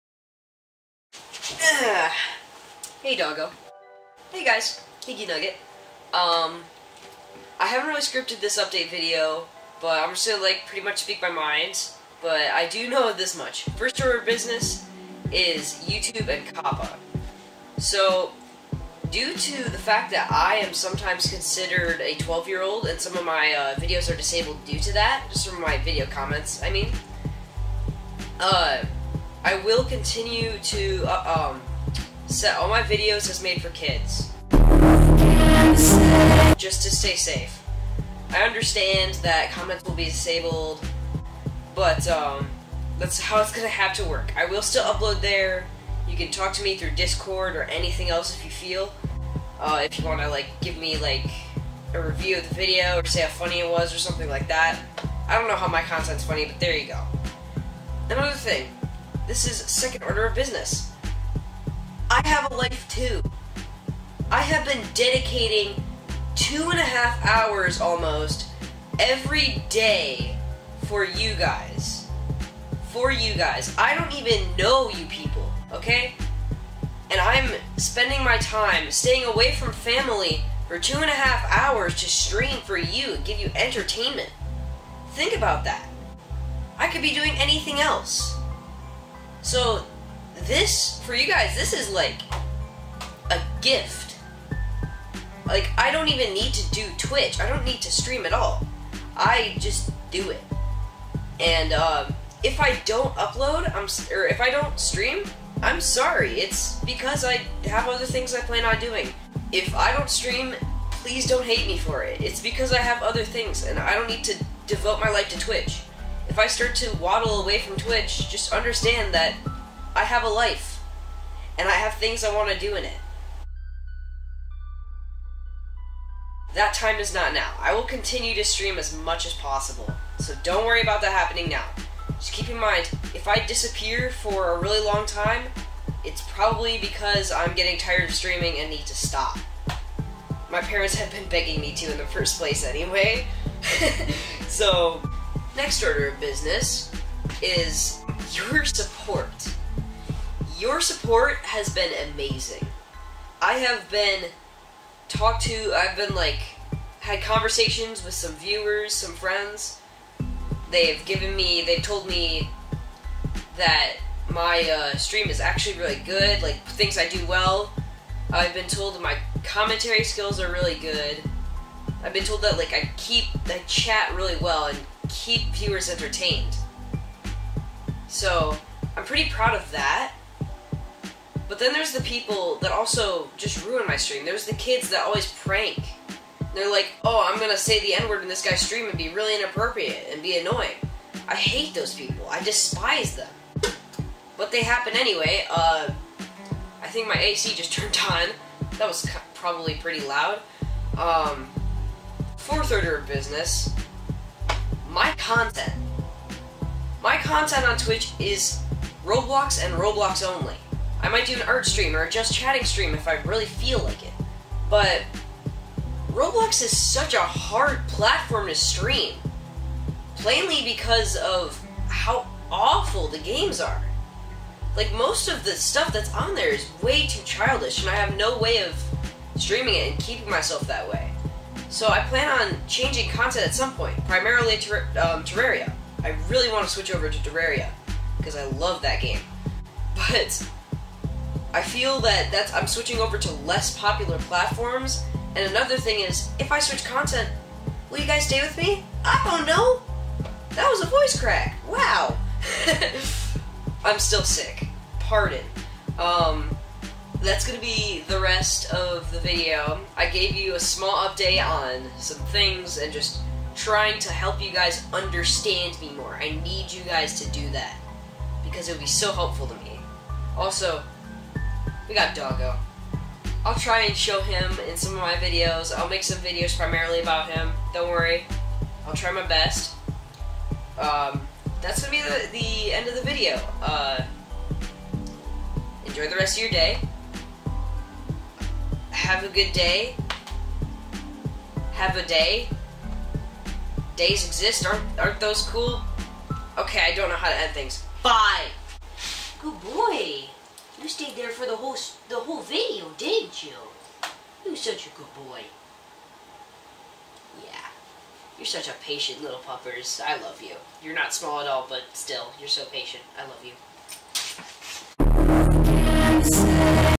I HAVE A LIFE TOO! (aka an update)
Music is by GlitchxCity & Enya

Here. Have an update video. I talk. (spoiler: there's a Doggo.)